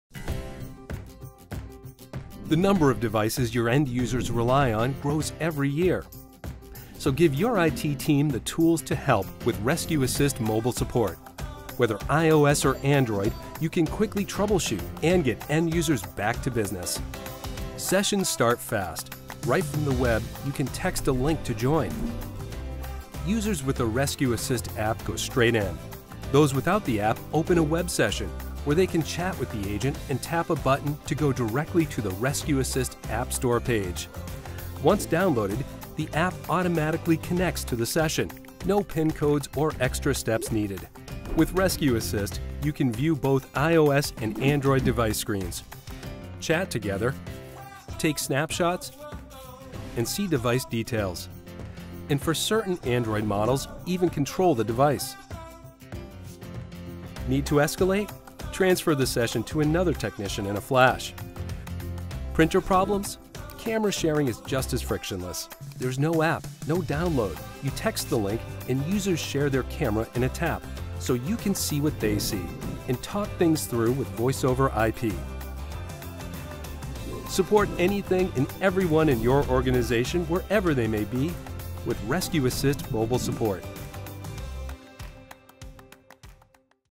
RescueAssist Mobile Support: On-The-Go Assistance For Any Device
With the increasing number of devices your customers and employees use every day, it’s critical to be able to support them from wherever they are. See how our RescueAssist Mobile Upgrade enables quick, frictionless mobile device support in this short video.